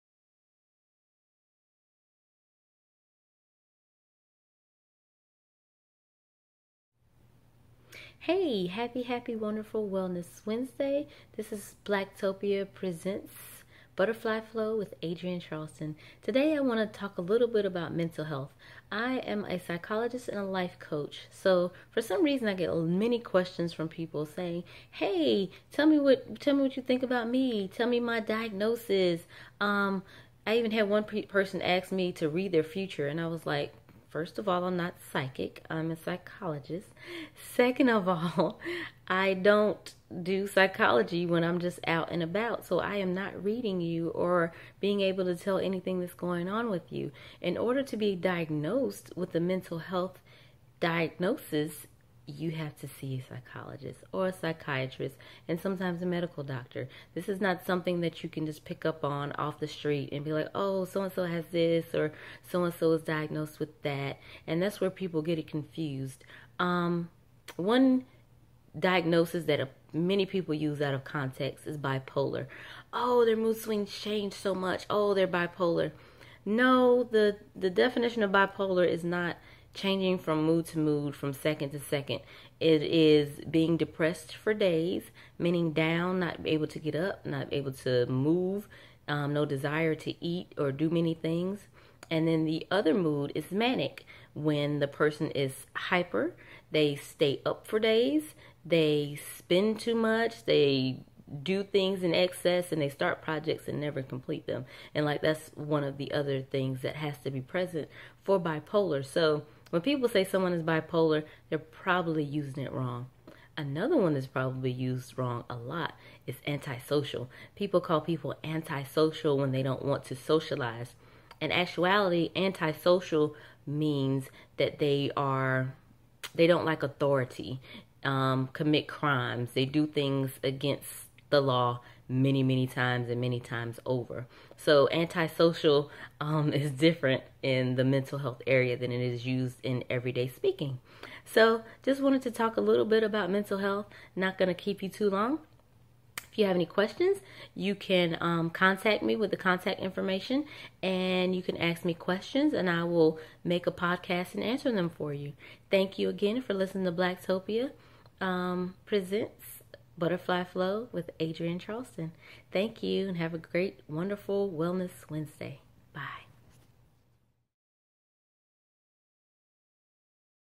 Butterfly Flow: Mental Health, Yo Ass NOT Bi-Polar OR Crazy... this is Why.
In this addition of Butterfly Flow with Adrienne Charleston she talks about the signs of someone being bi-polar, and having a mental illness. sometimes people misread themselves.